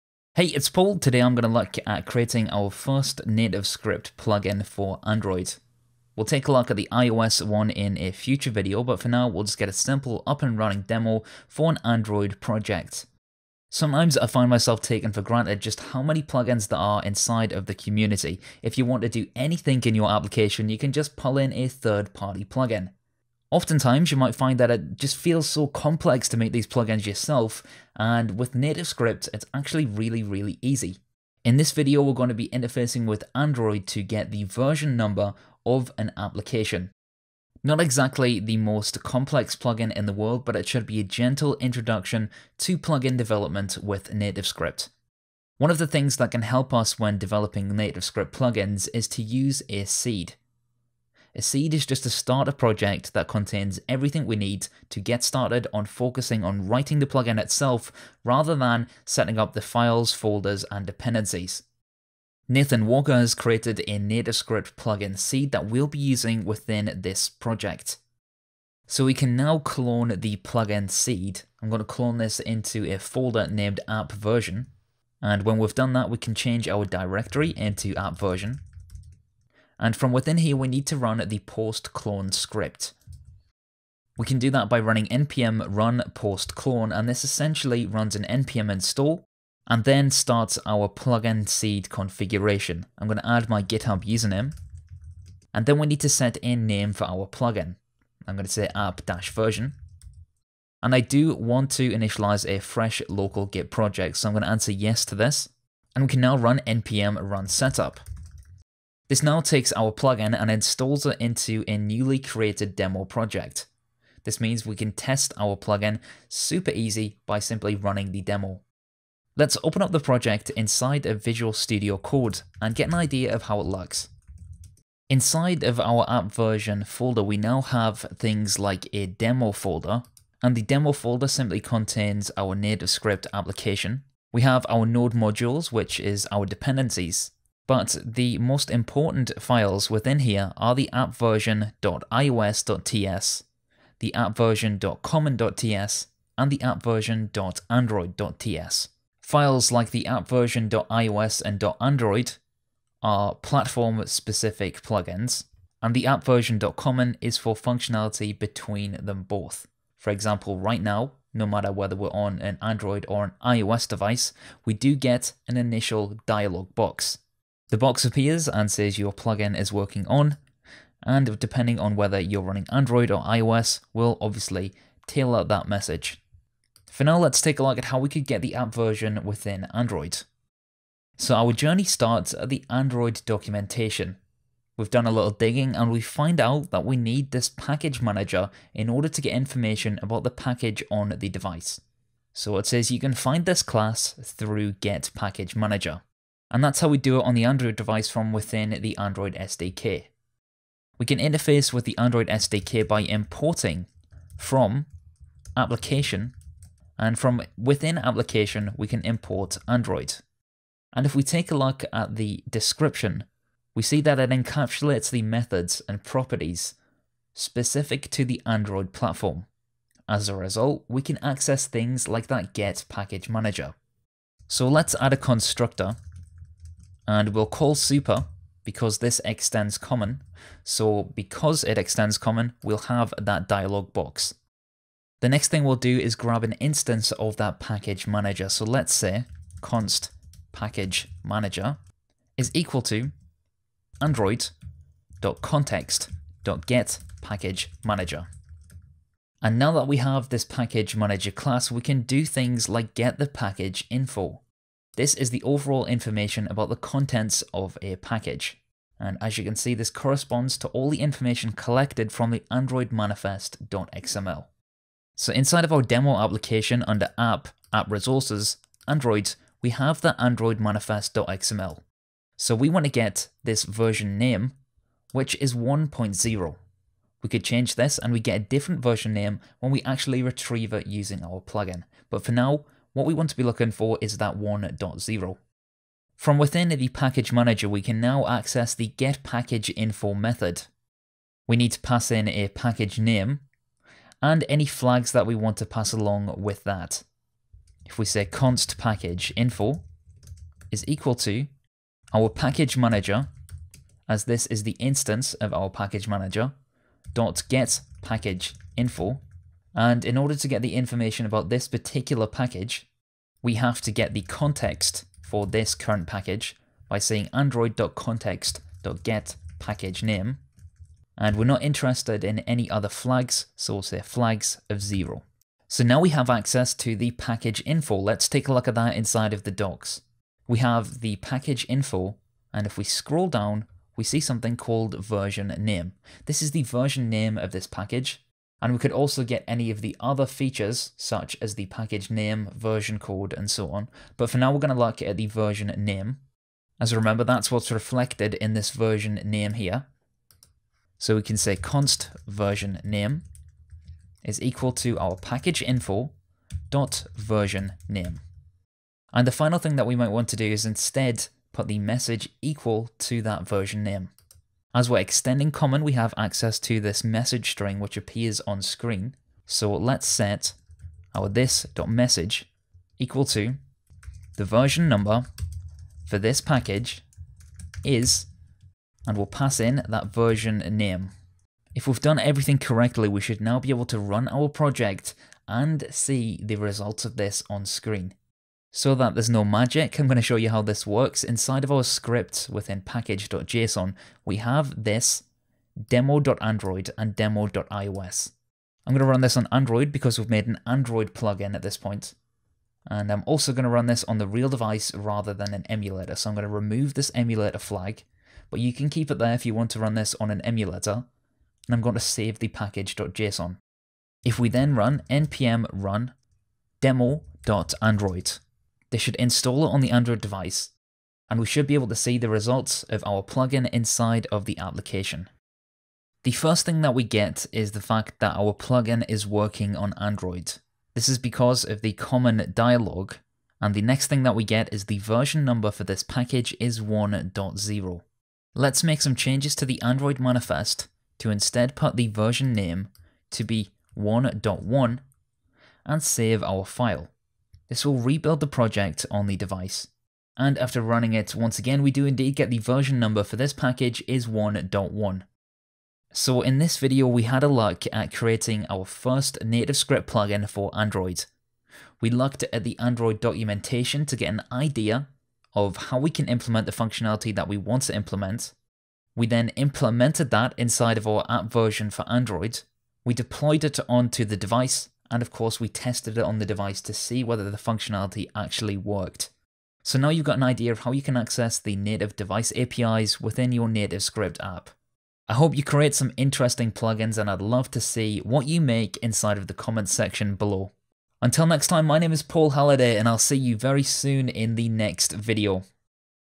A Gentle Introduction to NativeScript Plugin Development
In this video, we make our first NativeScript plugin for Android. We take a look at building it from Nathan Walker's seed. After which, we ensure to visit the Android documentation to reflect the development process.